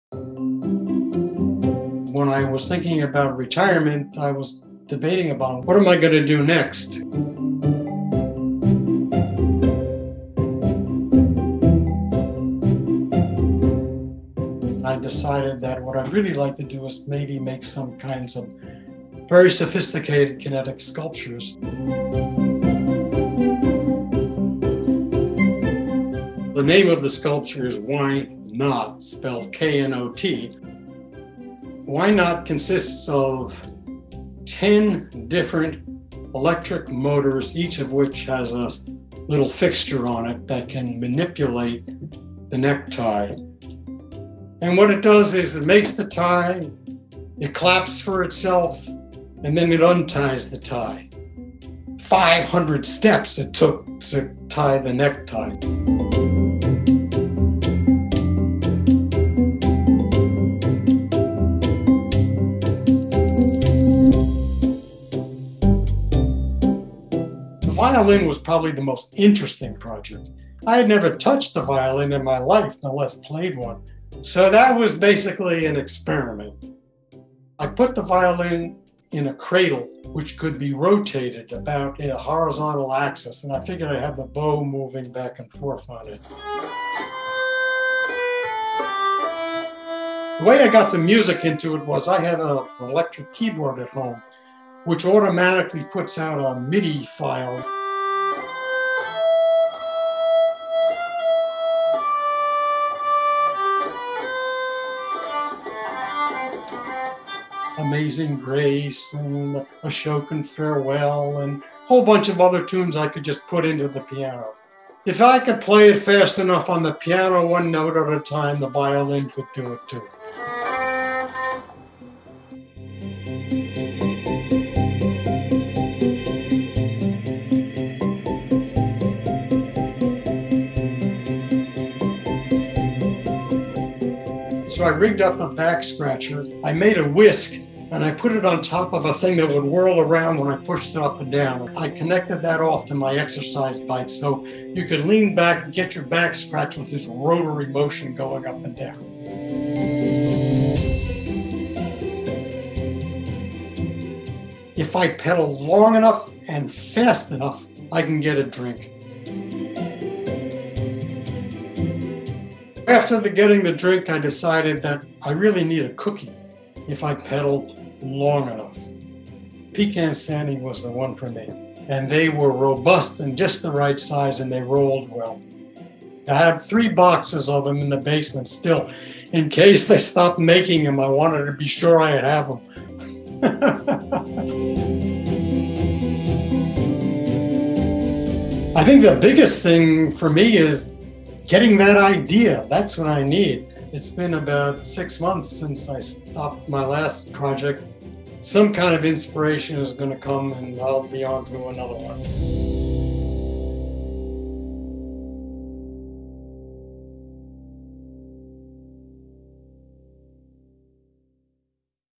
Retired Mechanical Engineer Puts Hundreds of Parts to Use in Kinetic Sculptures
Read more about the kinetic sculptures created by Seth Goldstein ’61, SM ’62, SM ’63, SCD ’66 on the Slice of MIT blog

After 31 years engineering medical solutions for the National Institutes of Health (NIH), Seth Goldstein ’61, SM ’62, SM ’63, SCD ’66 launched a second career as a sculptor—even landing him a place in a 2008 exhibit at American Visionary Art Museum in Baltimore.

Since retiring, Goldstein has completed four such multi-year projects, and not only do they move, as by definition kinetic sculptures must—they all achieve a specific goal.